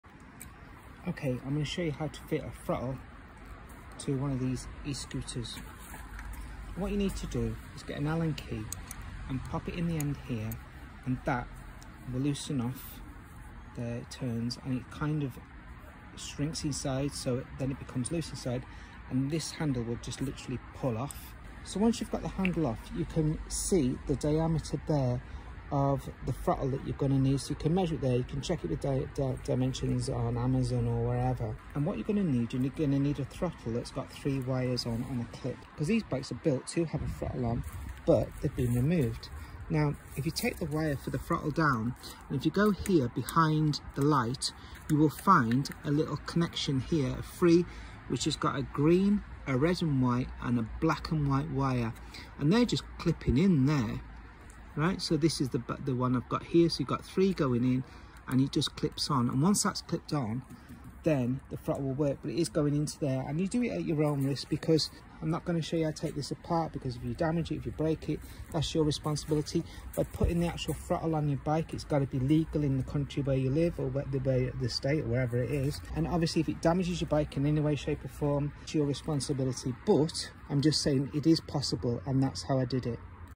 How to fit a throttle to an Eskuta 250w electric bike
Eskuta MK3 electric bike adaptations
How to fit a frottle to an electric bike